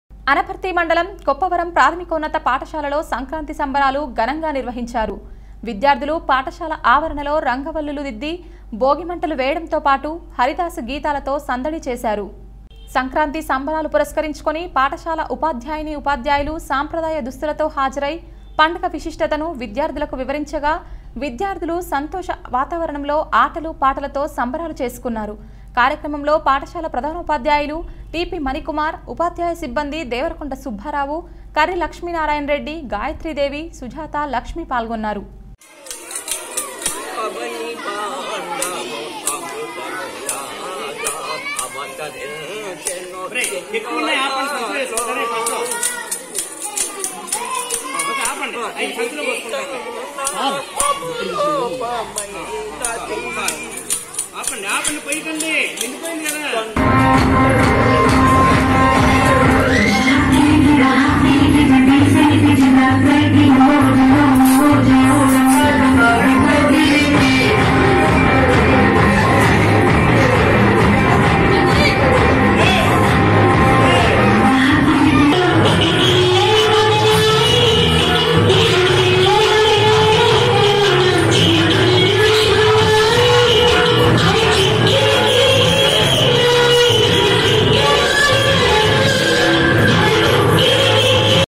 08-01-2022 Anaparthi ABC News East Godavari
అనపర్తి మండలం కొప్పవరం ప్రాథమికోన్నత పాఠశాలలో సంక్రాంతి సంబరాలు ఘనంగా నిర్వహించారు. విద్యార్థులు పాఠశాల ఆవరణలో రంగవల్లులు దిద్ధి భోగి మంటలు వేయడంతో పాటు హరిదాసు గీతాలతో సందడి చేశారు.
సంక్రాంతి సంబరాలు పురస్కరించుకుని పాఠశాల ఉపాధ్యాయిని ఉపాధ్యాయులు సాంప్రదాయ దుస్తులతో హాజరై పండగ విశిష్టతను
విద్యార్థులకు వివరించగా విద్యార్థులు సంతోష వాతావరణంలో ఆటలు, పాటలతో సంబరాలు చేసుకున్నారు. కార్యక్రమంలో పాఠశాల ప్రధానోపాధ్యాయులు టి. పి. మణికుమార్, ఉపాధ్యాయ సిబ్బంది దేవరకొండ సుబ్బారావు, కర్రి లక్ష్మీ నారాయణ రెడ్డి, గాయత్రీదేవి, సుజాత, లక్ష్మి పాల్గొన్నారు